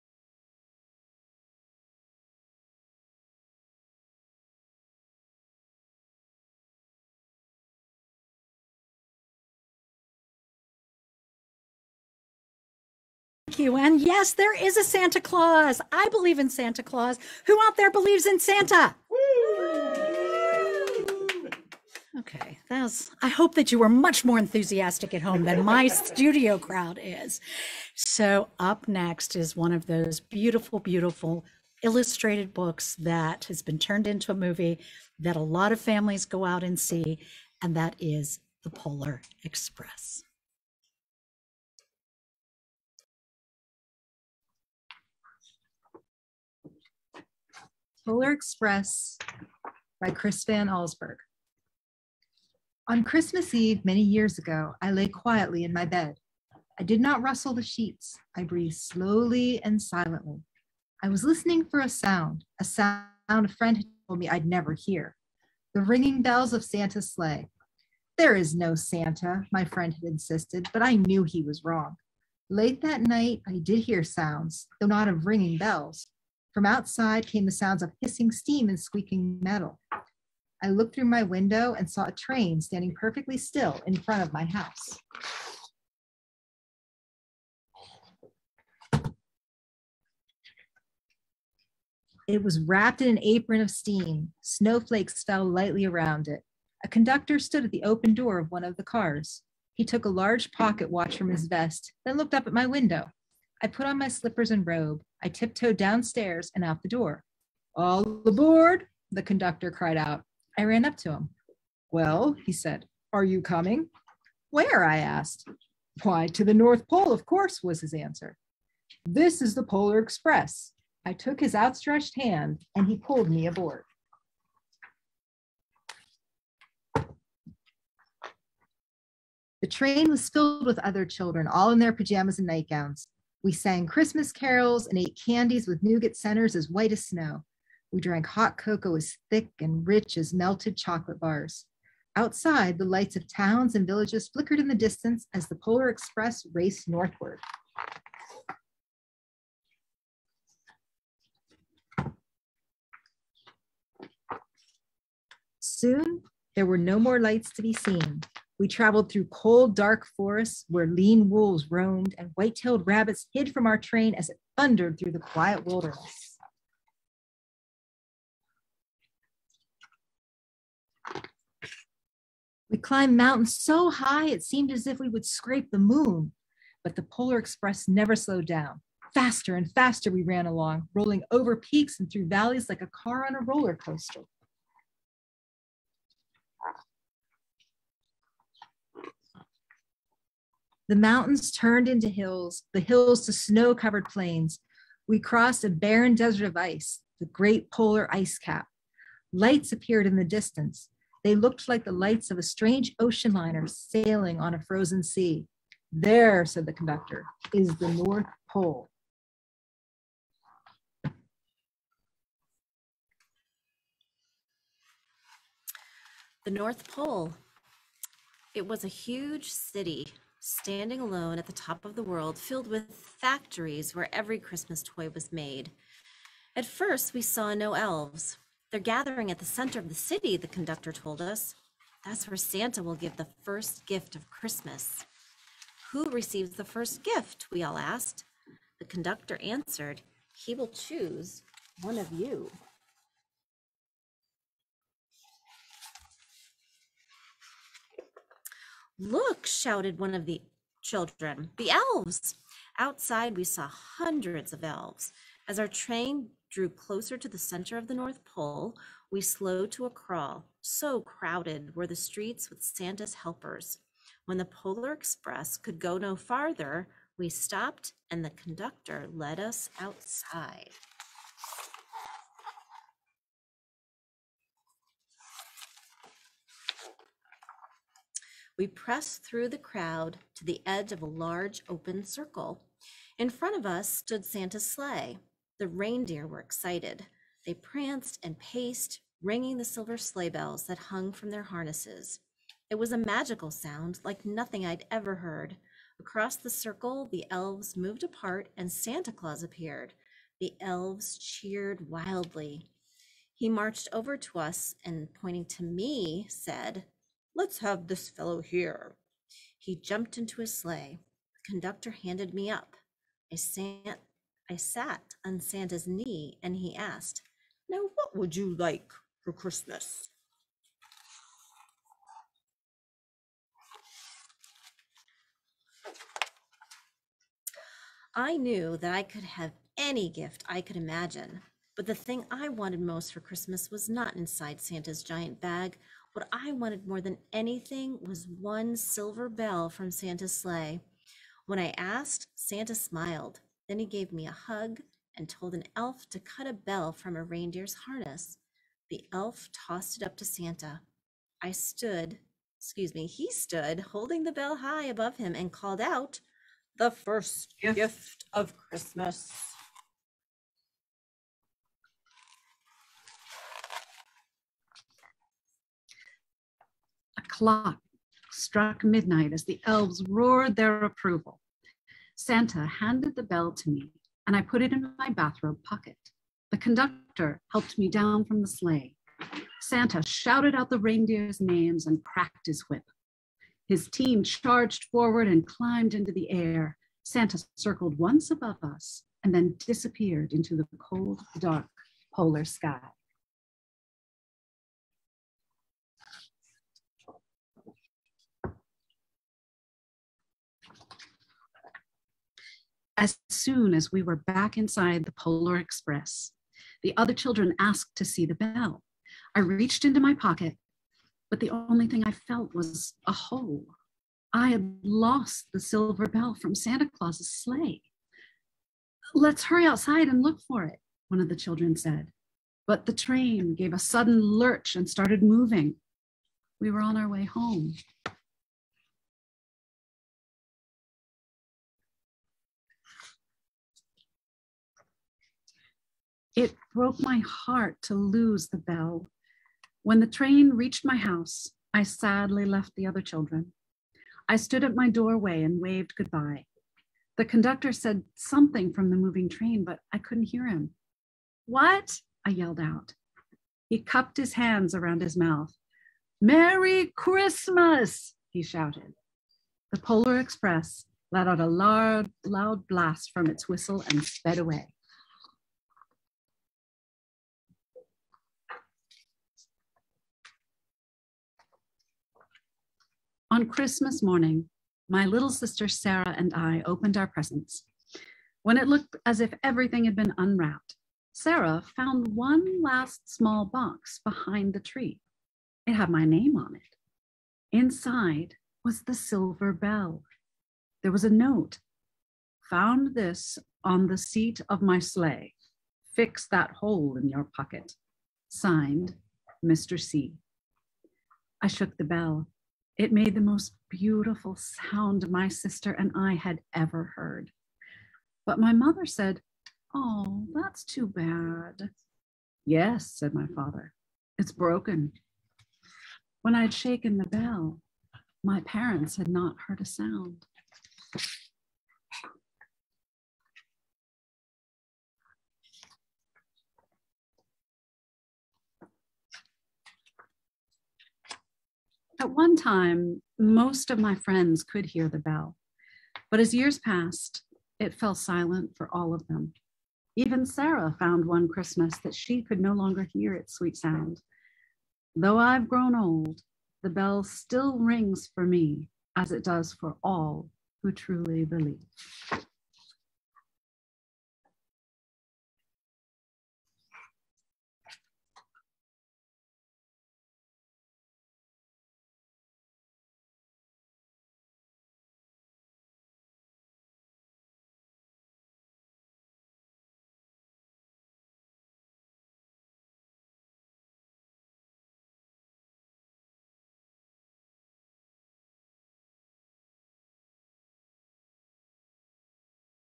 The Polar Express
Listen to Lori Wilson, Grace Irwin, and Carrie Marcinkevage reading the classic Polar Express.